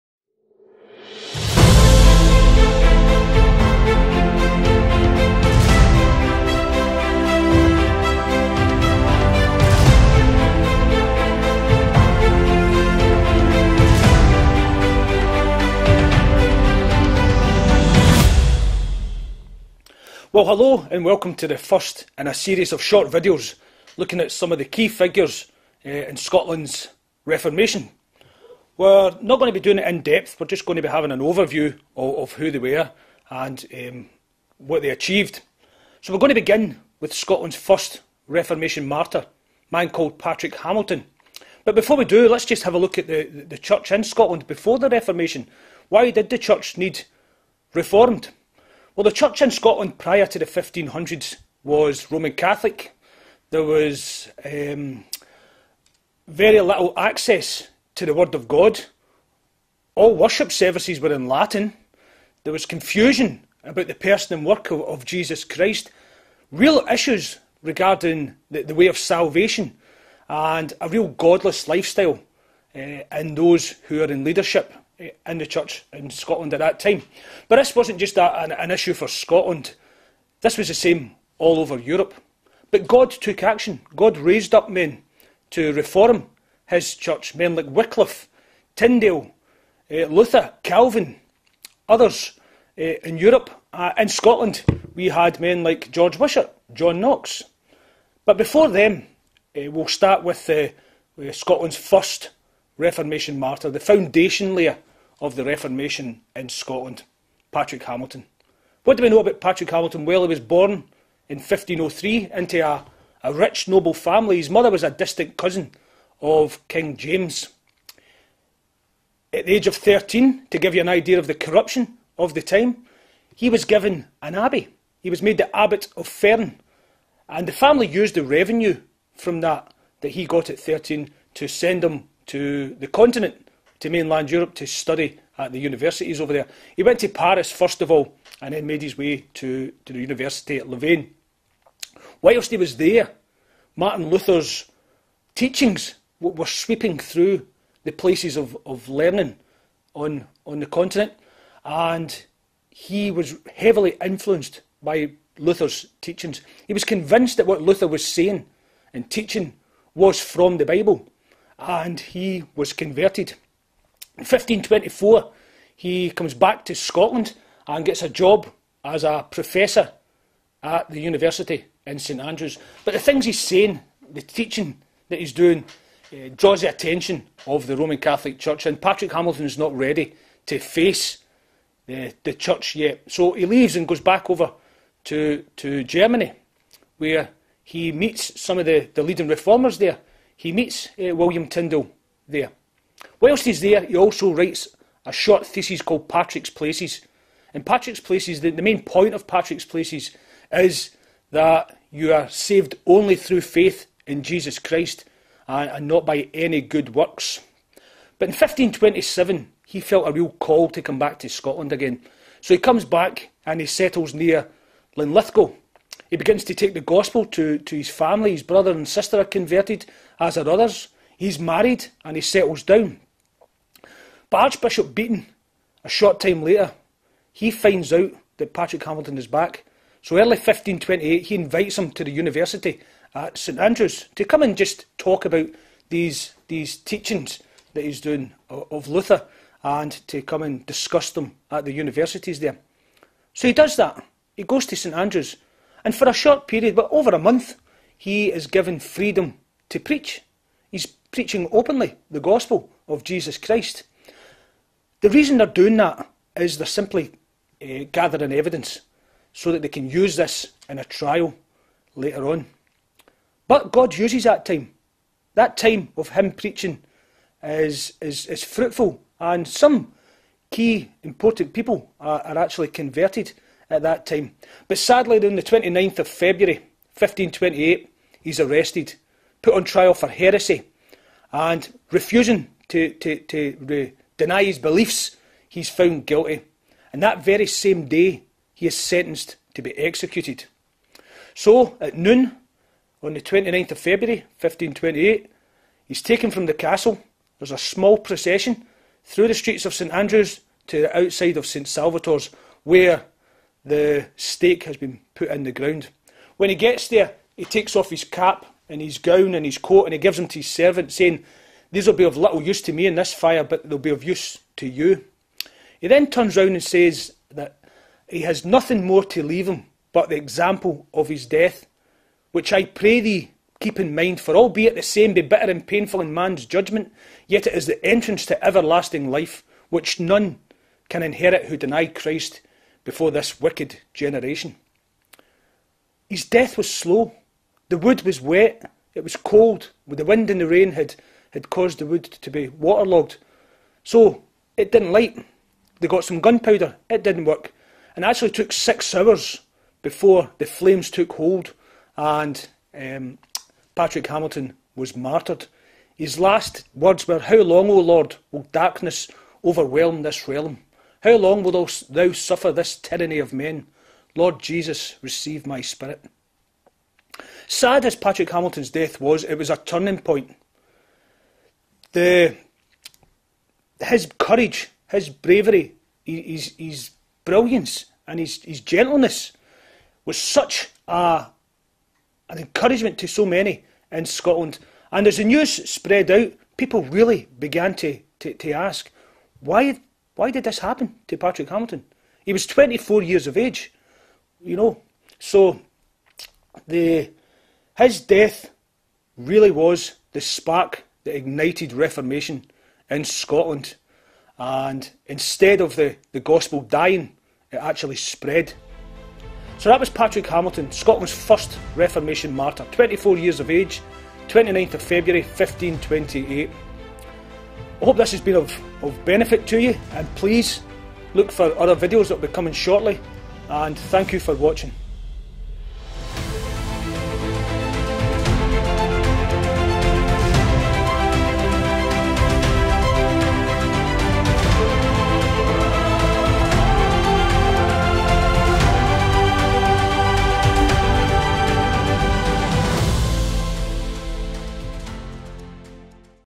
Exploring Scotland's Forgotten History - Patrick Hamilton
Exploring Scotland's Forgotten History from the start of the Reformation with  Jimmy Fisher of Scottish Reformation Tours. We begin with Patrick Hamilton the first preacher of the gospel in the Scottish Reformation and the first martyr.